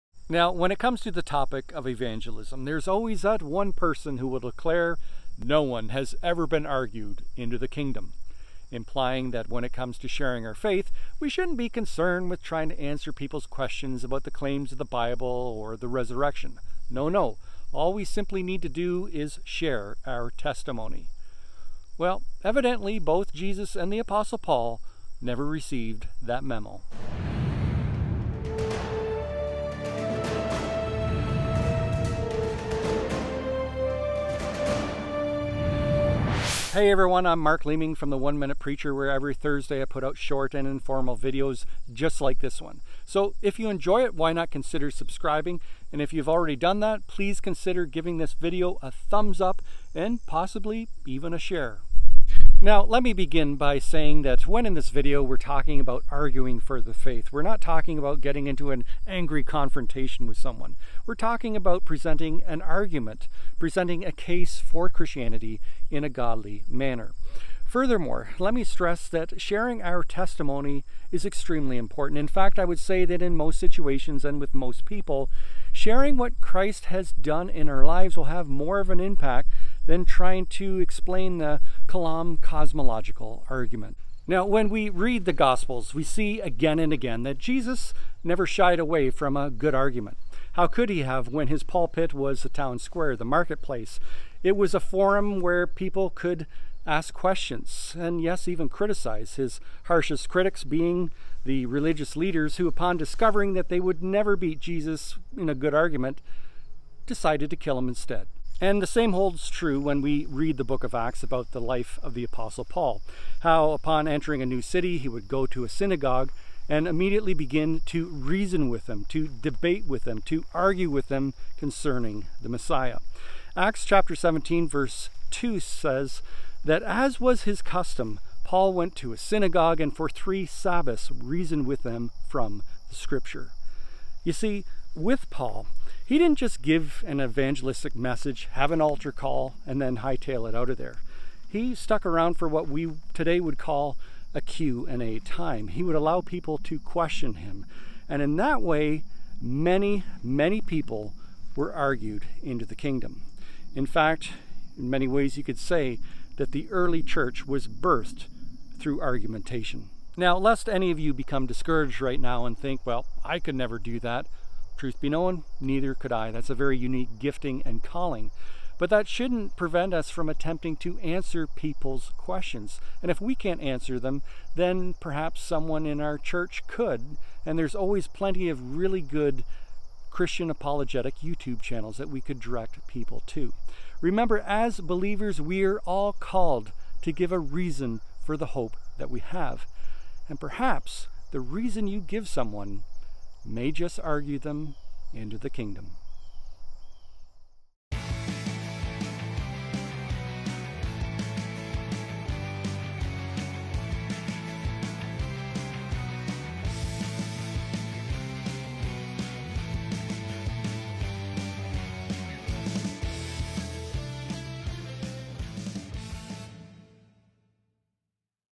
No One Has Been Argued Into The KIngdom?
Some claim that no one can be argued into the Kingdom. Both Jesus and Paul never thought that!